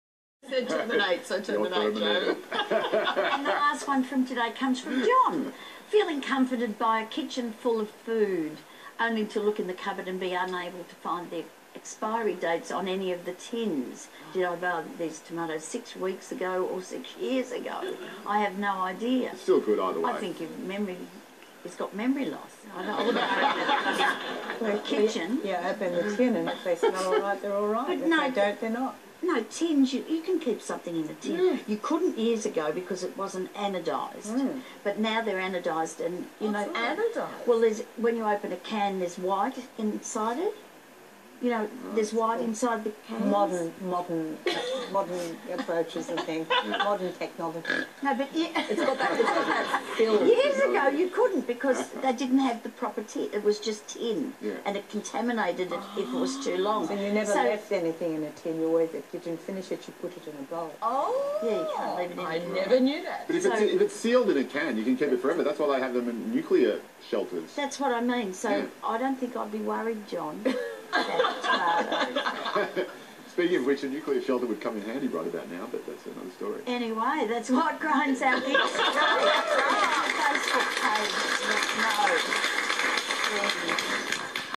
What grinds my gears segment, I got roasted by Denise Drysdale about canned food
Made a comment on the internet. Got briefly roasted by Denise Drysdale which is an honor.